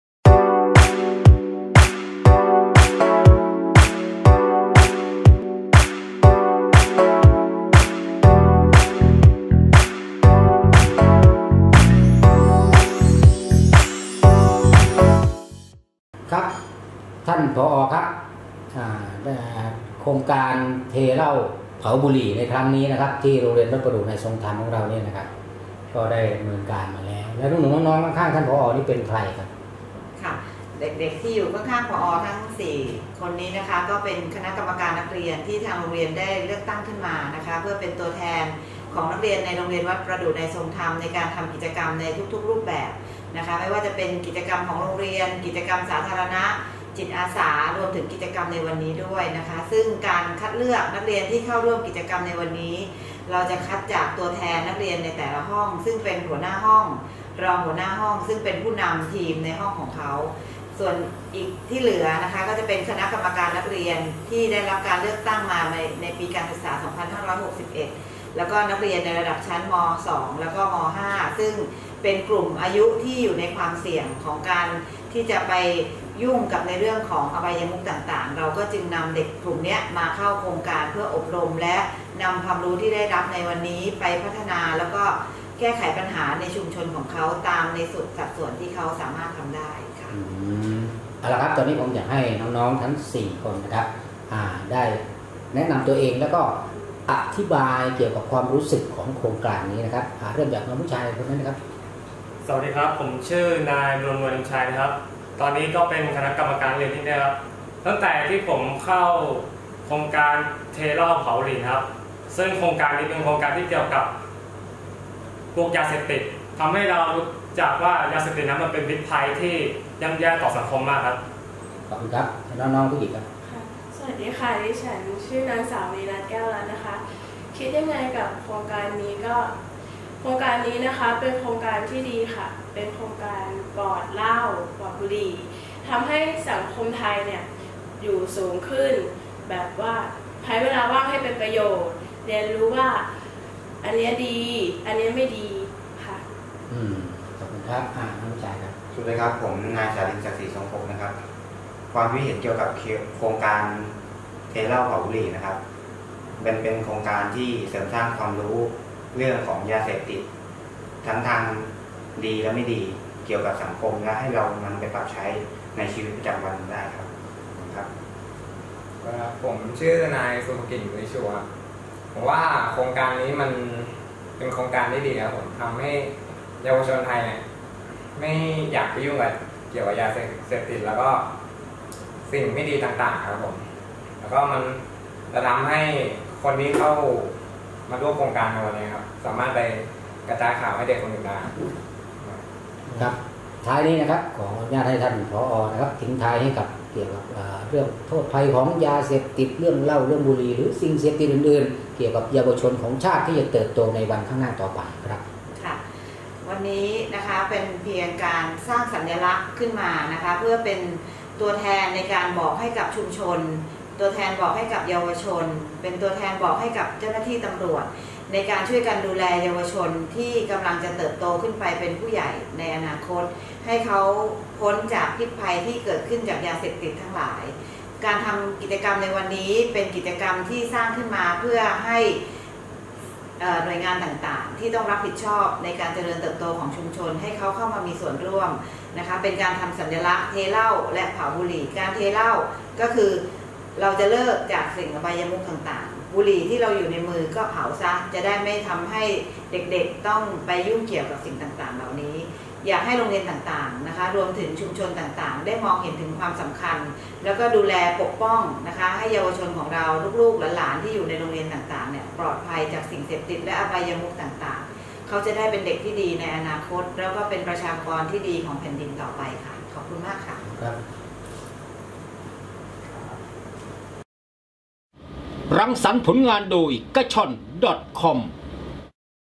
โครงการสร้างภูมิคุ้มกันและป้องกันยาเสพติดในสถานศึกษา 05
((หนังสือพิมพ์กะฉ่อนดอทคอม ทีมงาน วิทย์ วัดอรุณ))
พล.ต.ต.บุญญฤทธิ์ รอดมา ผู้บังคับการตำรวจนครบาล 7 เปิดโครงการสร้างภูมิคุ้มกันและป้องกันยาเสพติดในสถานศึกษา ที่โรงเรียนวัดประดู่ในทรงธรรม (มีคลิป)